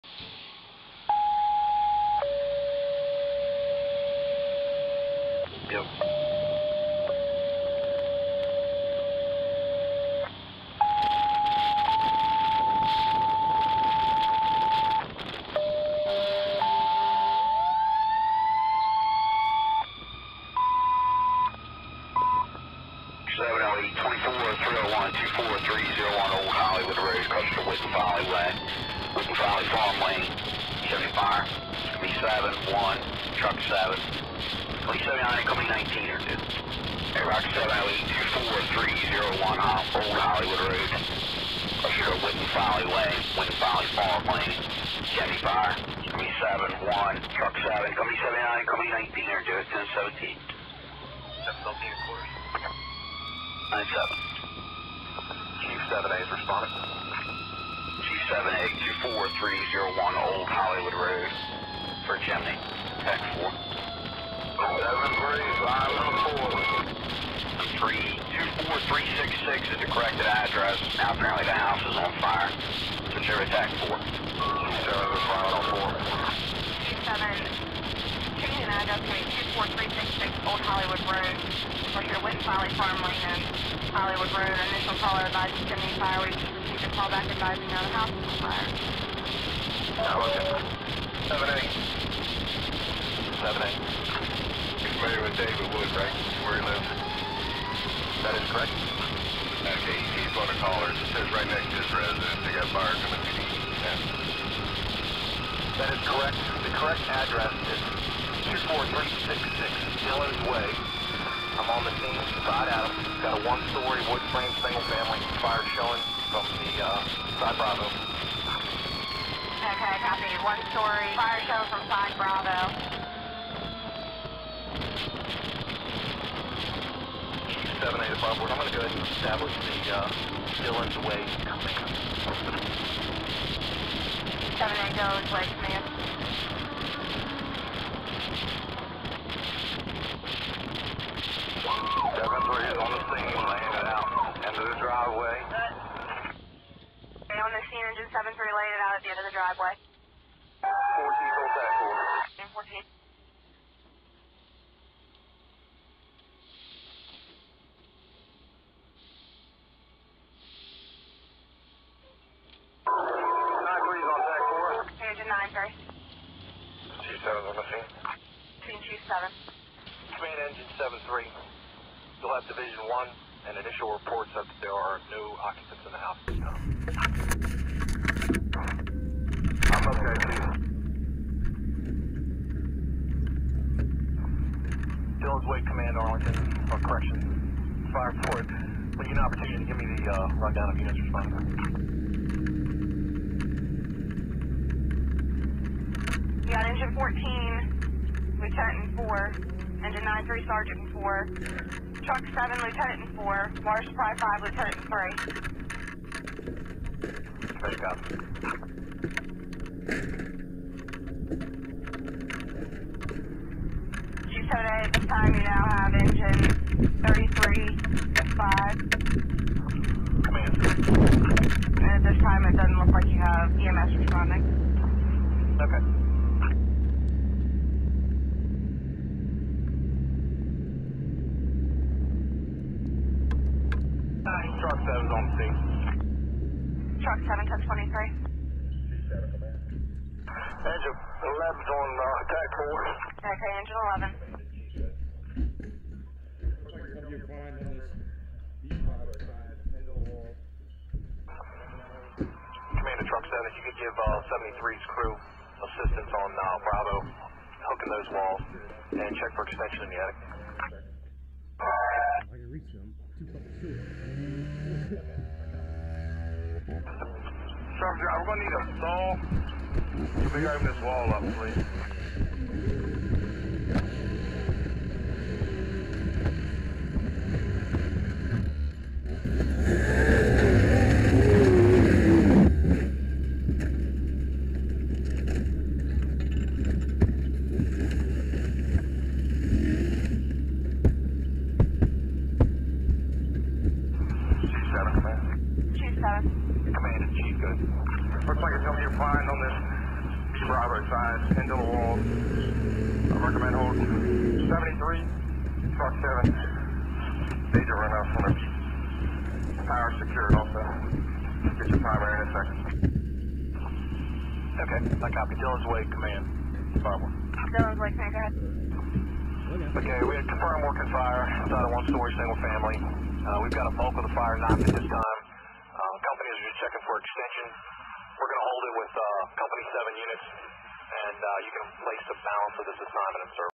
Dylan's Wy House Fire
Hollywood Volunteers were dispatched for a Chimney Fire off of Old Hollywood Road in Hollywood Maryland.  Assignment was upgraded to a House Fire with reports of fire coming from the eve's of the house.  Crews arrived finding fire on the "Bravo" side of the structure and made quick work of the fire in the walls and exterior of the structure.  The assignment was scaled back to the units from Hollywood and returned the rest.